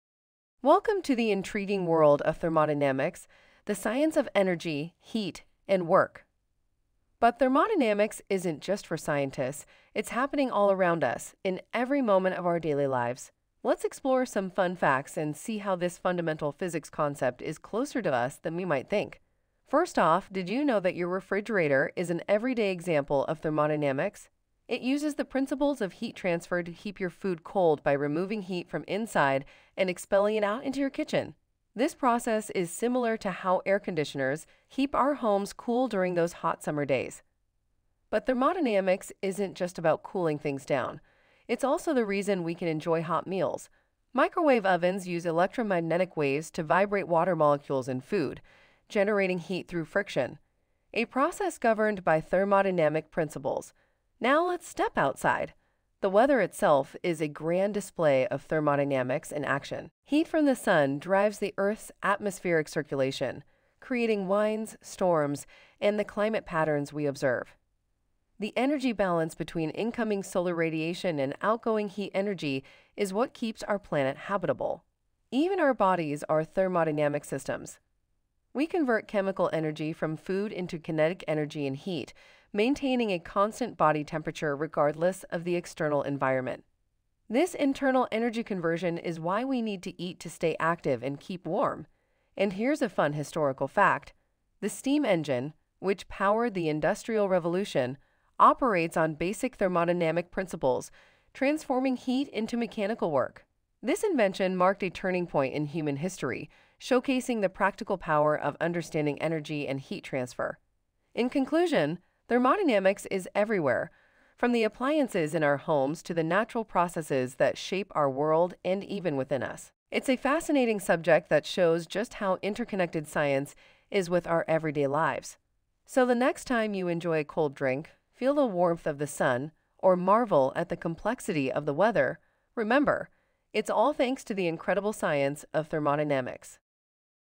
Unlocking the Mysteries of Thermodynamics!
Thermodynamics is a branch of physics that deals with the relationships between heat and other forms of energy. In essence, it examines how energy is converted from one form to another and how it affects matter. Thermodynamics is fundamental in a wide range of scientific and engineering disciplines, including chemical engineering, mechanical engineering, and physical chemistry, because it helps us understand and predict how systems will behave in real-world situations.

The core of thermodynamics is encapsulated in its four laws:

Zeroth Law of Thermodynamics: This law states that if two systems are each in thermal equilibrium with a third system, then they are in thermal equilibrium with each other. This law establishes temperature as a fundamental and measurable property of matter.

First Law of Thermodynamics: Also known as the law of energy conservation, it states that energy cannot be created or destroyed in an isolated system. The total energy of an isolated system is constant. Energy can be transformed from one form to another, and work can be done by or on the system, but the total energy remains unchanged.

Second Law of Thermodynamics: This law introduces the concept of entropy, stating that in any closed system, the amount of entropy (disorder) will either remain constant or increase over time. In other words, energy transformations are not completely efficient, and some energy is always lost to disorder. It also implies that heat cannot spontaneously flow from a colder body to a hotter one.

Third Law of Thermodynamics: This law states that as the temperature of a system approaches absolute zero, the entropy of the system approaches a constant minimum. In practical terms, it is impossible to reach the absolute zero temperature (0 Kelvin).

These laws provide a framework for understanding the transfer of energy and work in any physical or chemical process. They have profound implications for such phenomena as the efficiency of engines and refrigerators, the direction of chemical reactions, and the behavior of gases and solids at different temperatures.

Thermodynamics intersects with other areas of physics and chemistry, such as statistical mechanics, which deals with the microscopic behaviors of particles in a system, and kinetics, which concerns the rates of chemical reactions. Through these intersections, thermodynamics offers a powerful set of tools for analyzing and designing chemical processes, energy systems, and even biological organisms, making it a cornerstone of modern science and engineering.